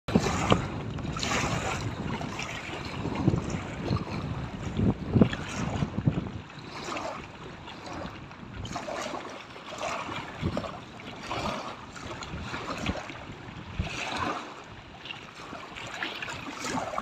Gans Lisa Küssnacht, September 2024
Unsere Gans in Küssnacht vor ein paar Wochen :-)